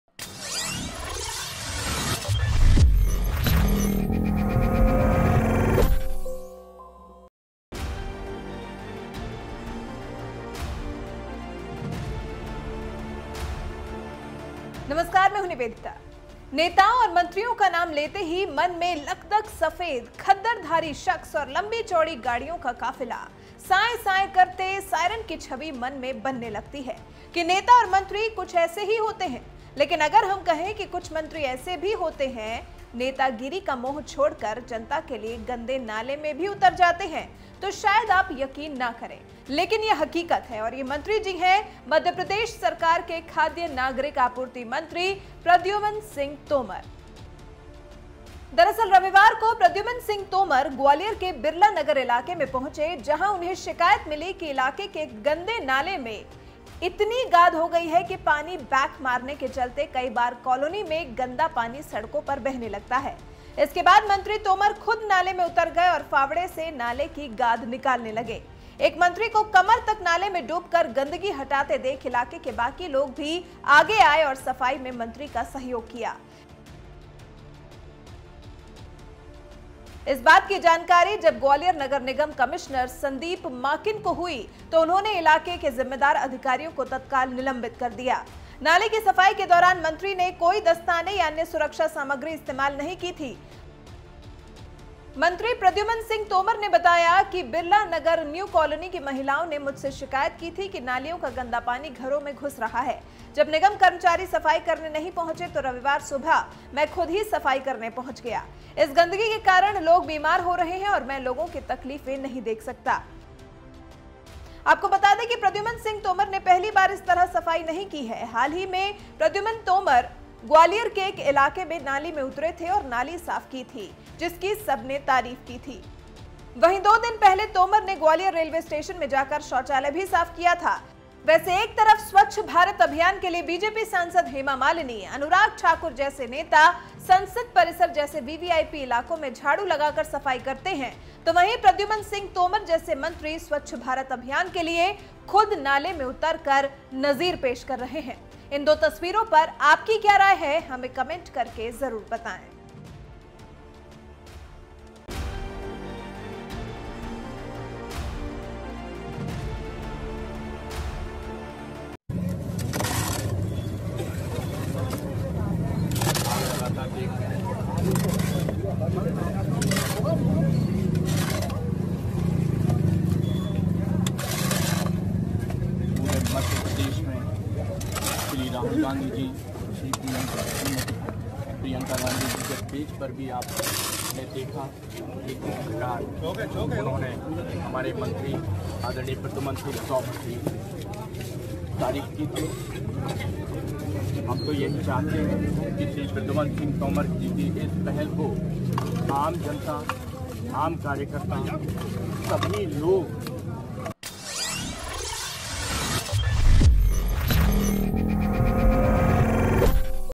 नेता हो तो ऐसा जो जनता के लिए नाले में भी उतर जाए
मध्य प्रदेश सरकार के एक ऐसे मंत्री जो सफाई करने के लिए खुद नाले में उतर गए, जानिए कौन हैं ये मंत्री जी !

Dheerendra
Krishan kant